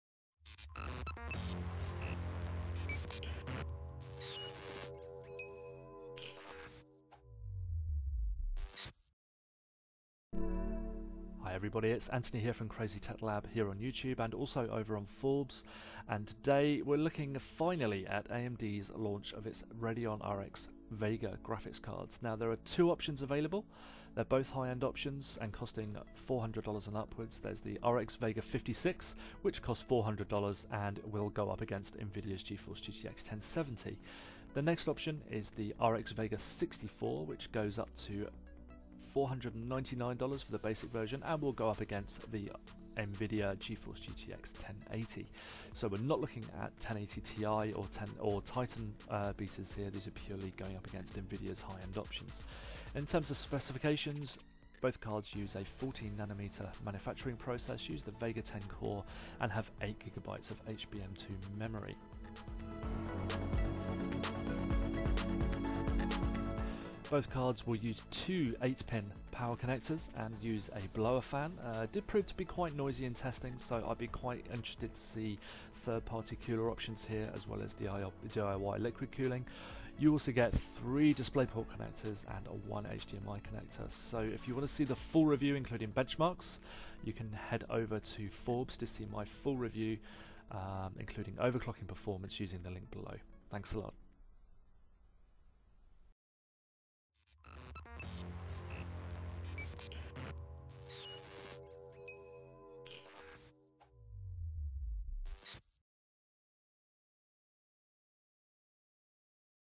AMD RX Vega Is Here!
See my full review of AMD's Vega 56 graphics card versus Nvidia's GeForce GTX 1070